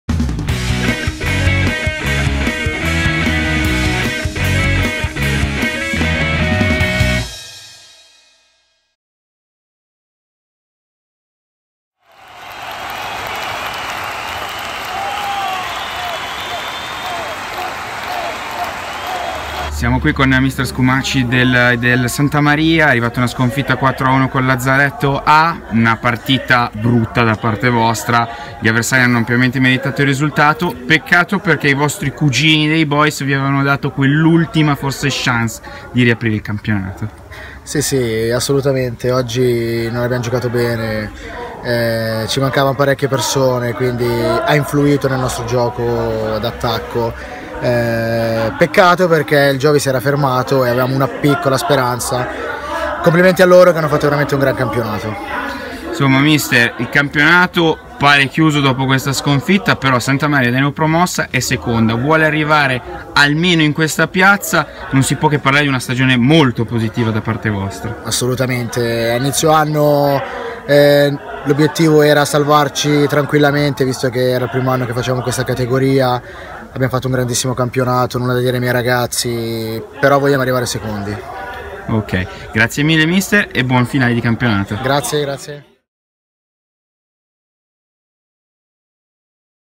Intervista S. Maria - Andrea Scumaci
Le parole dell'allenatore del S. Maria, Scumaci, dopo la sconfitta contro Lazzaretto.